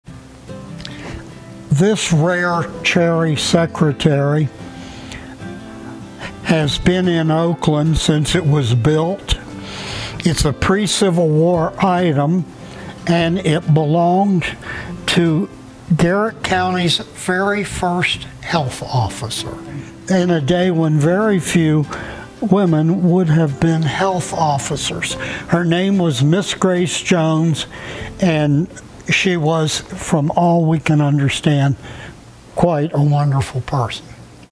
Garrett County Museums - Transportation Museum Second Floor - Health Officer Secretary Cabinet
Garrett County Health Officer's Cherry Secretary Cabinet from Pre-Civil War display on the 2nd floor of the Transportation Museum in Garrett County, Maryland.